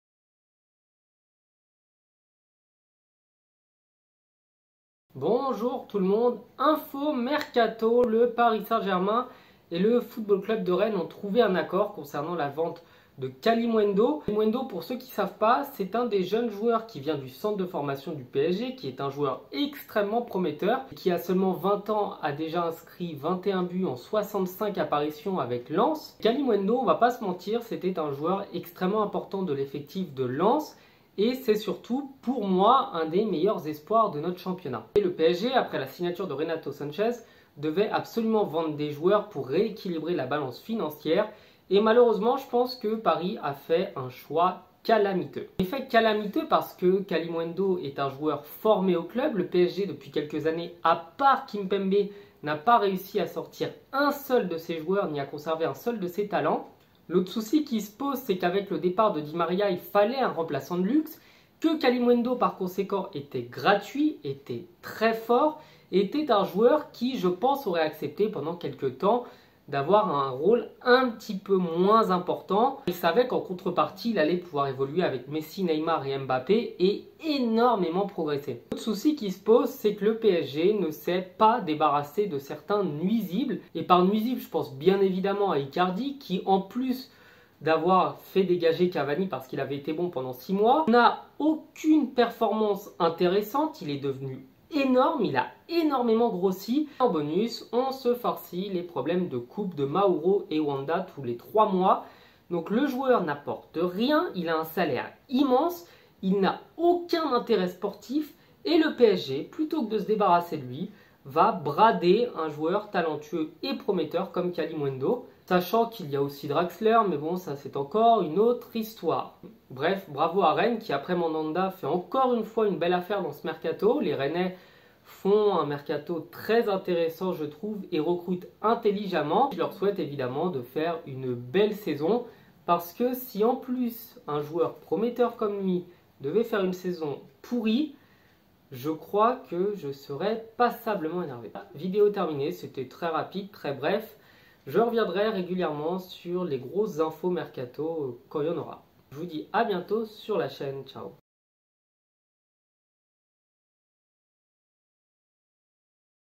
PARIS FAIT N'IMPORTE QUOI DANS LE MERCATO! KALUMUENDO VERS RENNES
Je ne suis pas du tout fan de la vente des jeunes... Paris était bien parti pourtant